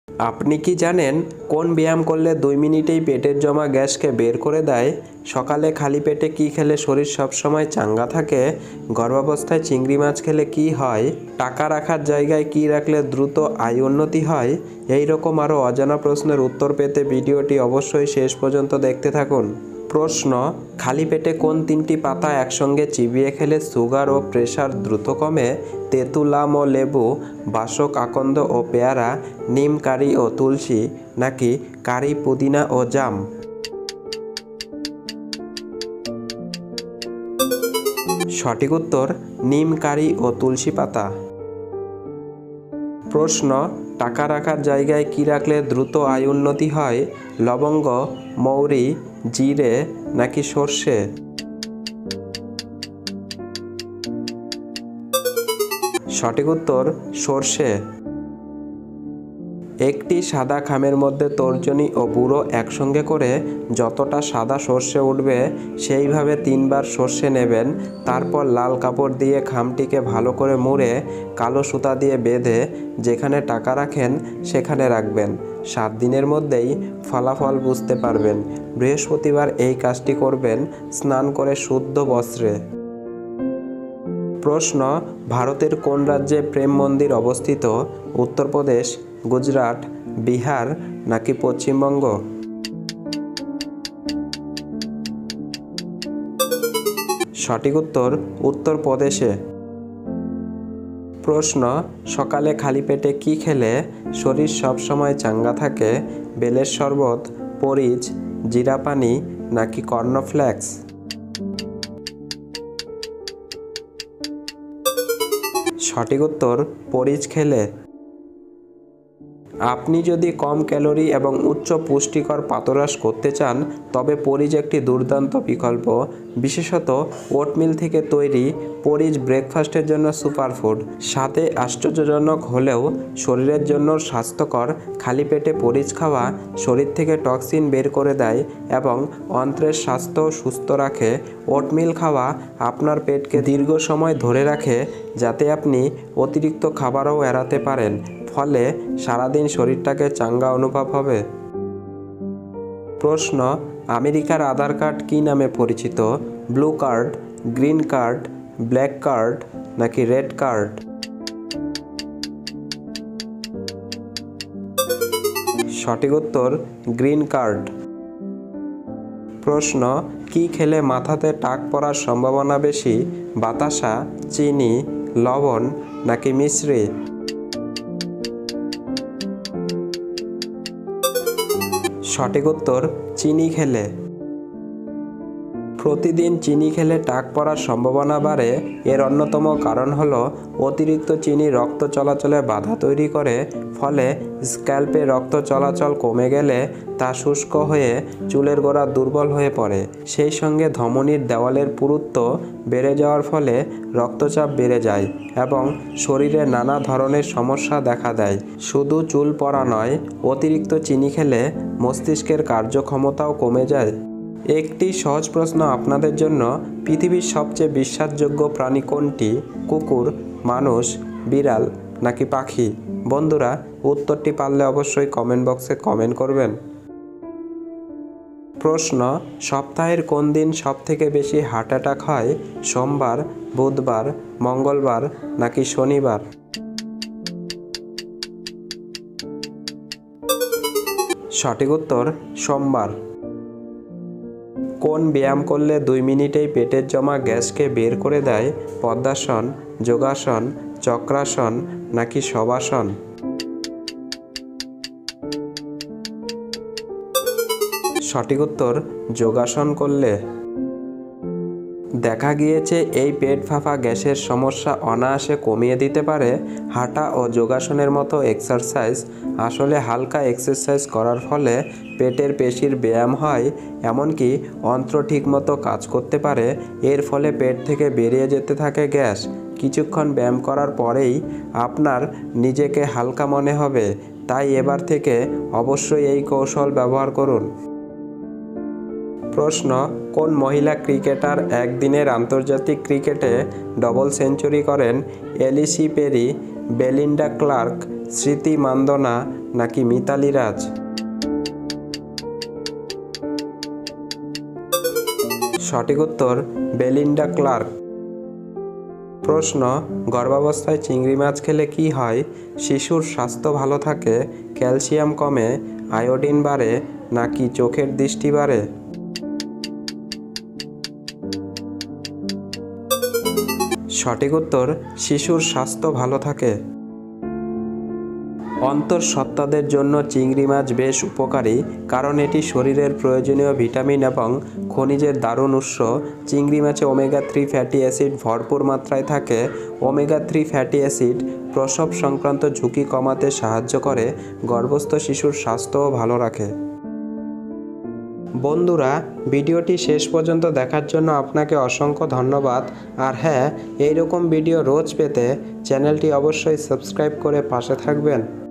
কোন ব্যায়াম করলে ২ মিনিটেই পেটের জমা গ্যাস বের হয়ে যায় ?
আজকের ভিডিওতে রয়েছে 10টি গুরুত্বপূর্ণ প্রশ্ন ও তার উত্তর,, তাই ভিডিওটি শেষ পর্যন্ত দেখার জন্য অনুরোধ রইল